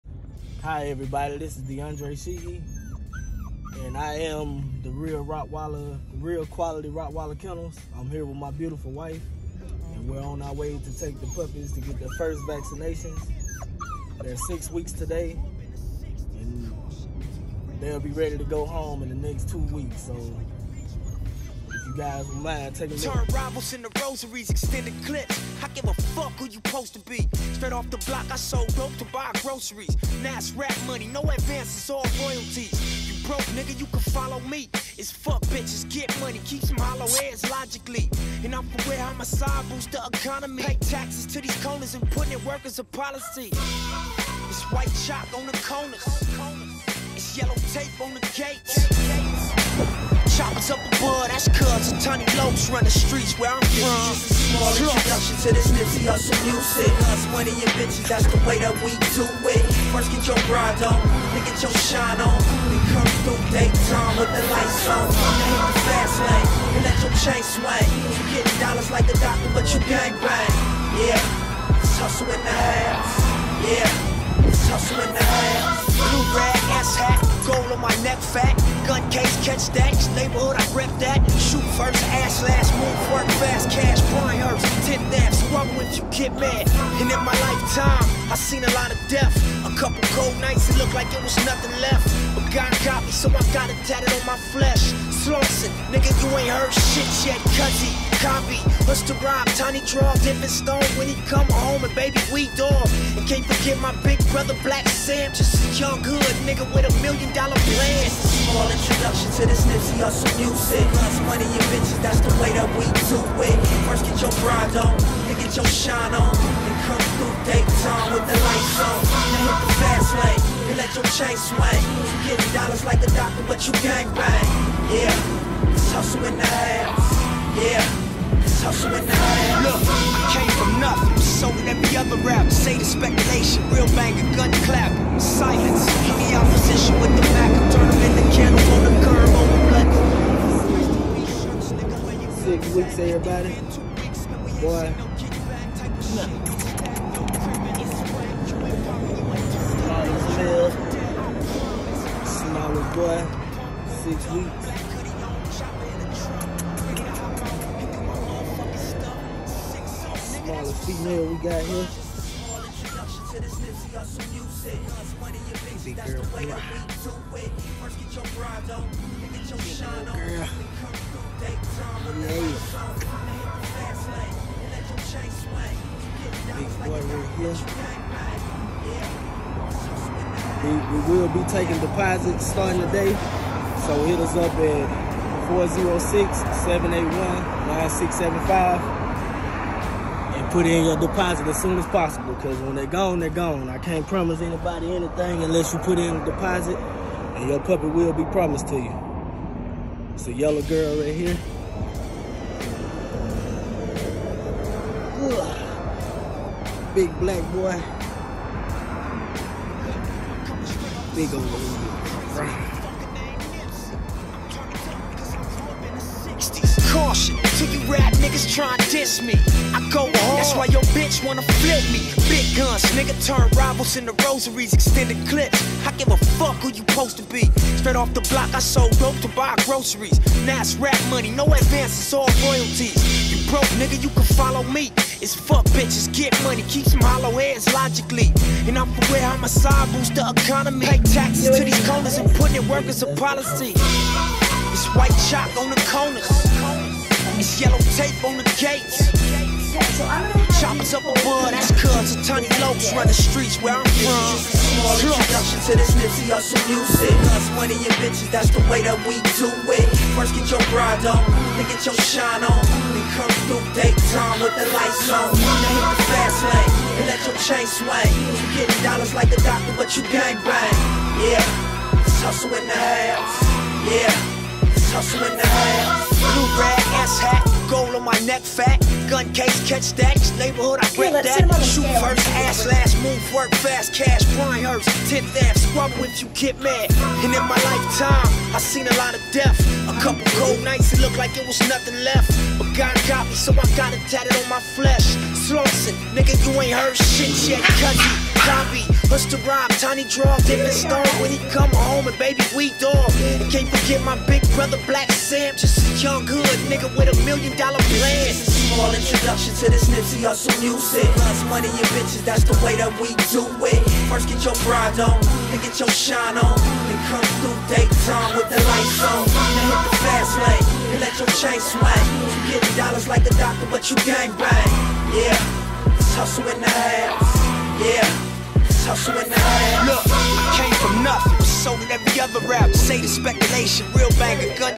Real Quality Rottweiler Kennels
6 week old Rottweiler puppies first vaccination and deworming meds. RQRK up and coming kennels